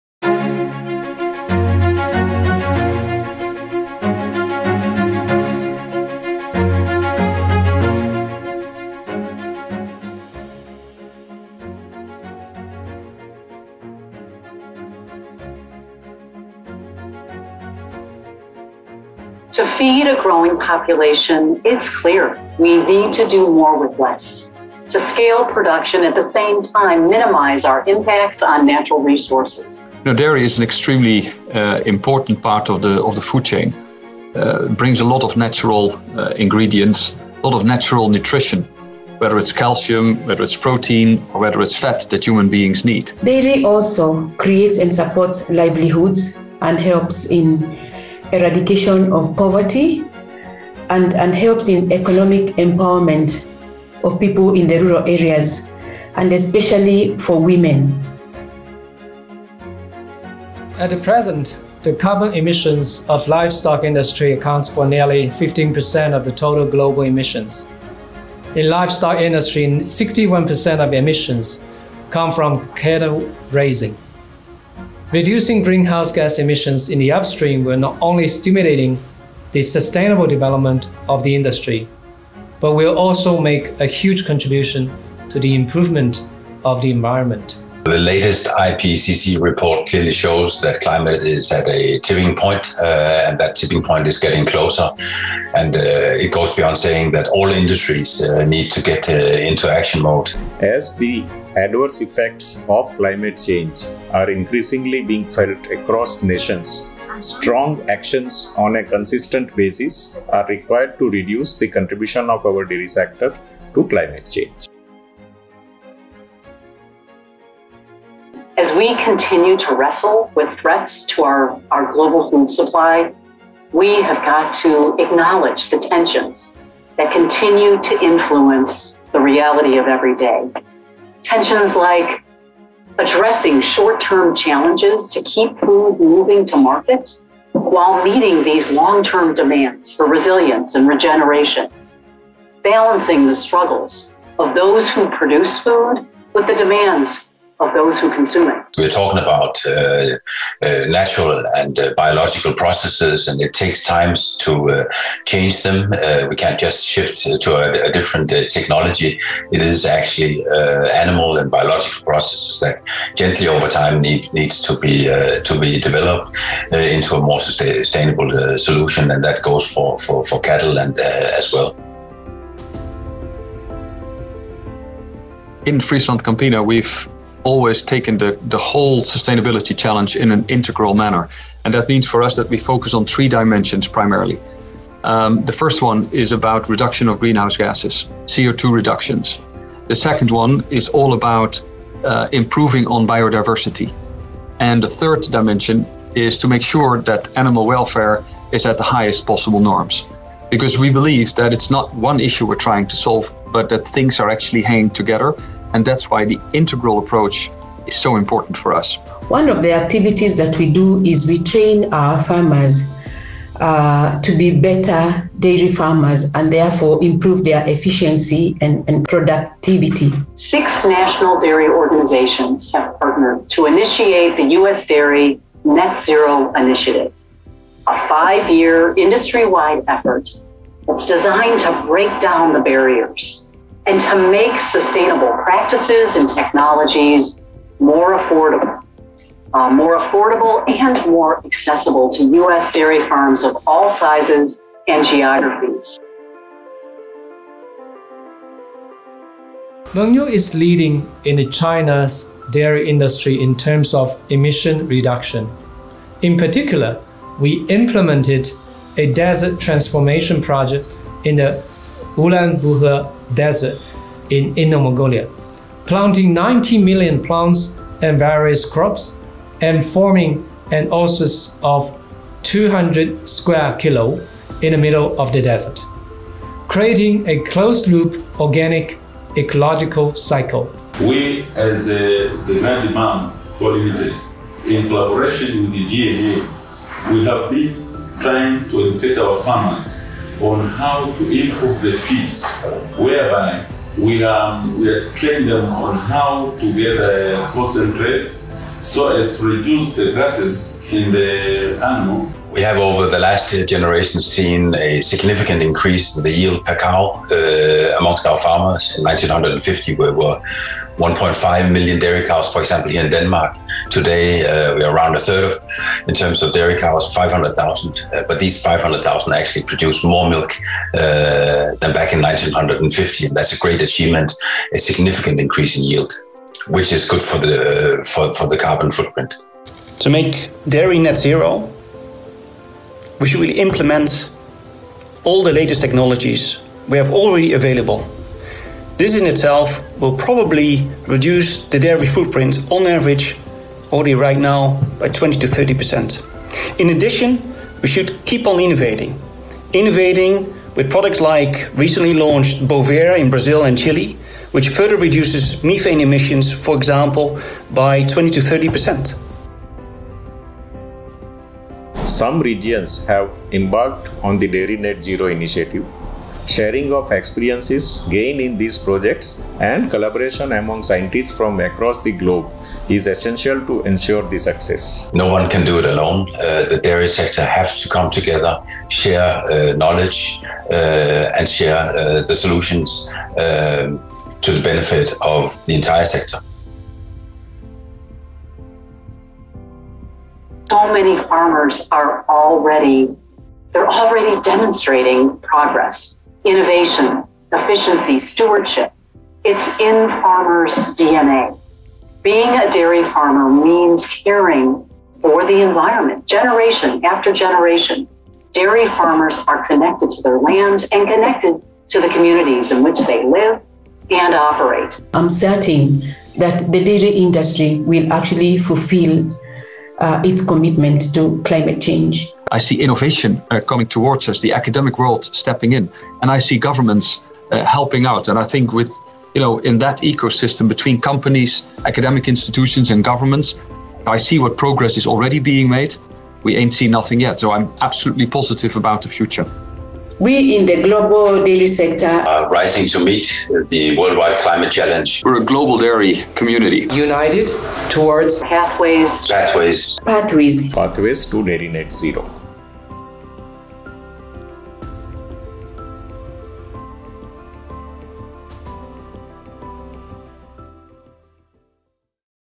Meet the dairy leaders making climate action commitments
Hear from the heads of leading dairy organizations about how they are rising to meet the global climate challenge and why they support Pathways to Dairy Net Zero, the first climate initiative of its kind in agriculture.
to find out more